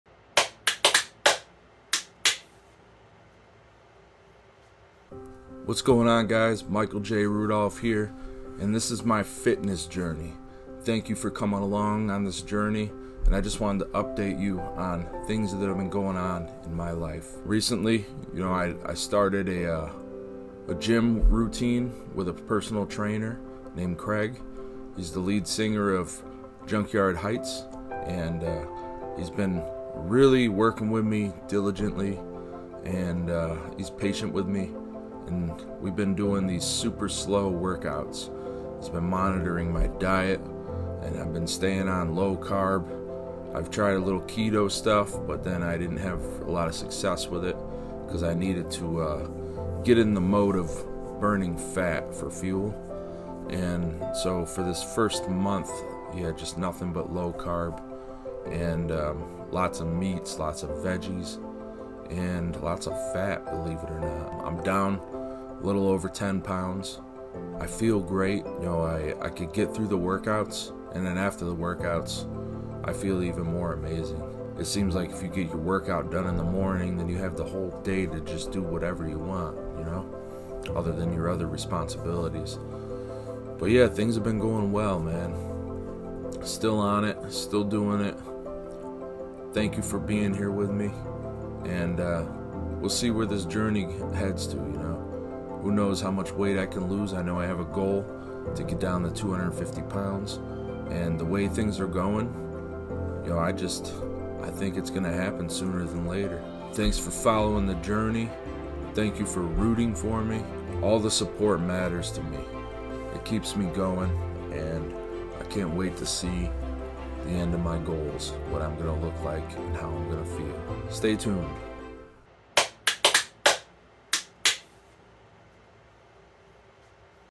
1 month in Fitness Journey 310lbs
One Month In. I am now at 310lbs
I want to get fit, lose weight, gain strength and live longer for myself and everyone in my life. Feel free to follow the journey and hold me accountable. I want this more than ever!